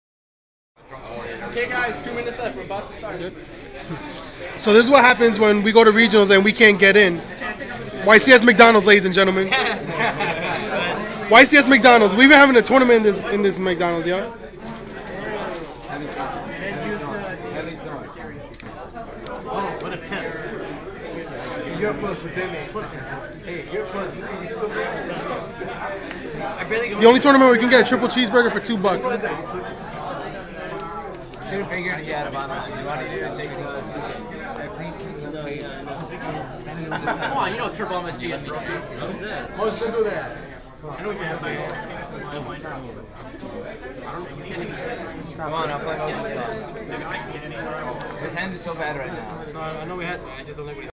Yu-Gi-Oh! - YCS McDonalds
This is what happens when King's Games doesn't have enough space for the amount of people that showed up to the Regional! The people that couldn't make it inside made their own tournament...at MCDONALDS?!?!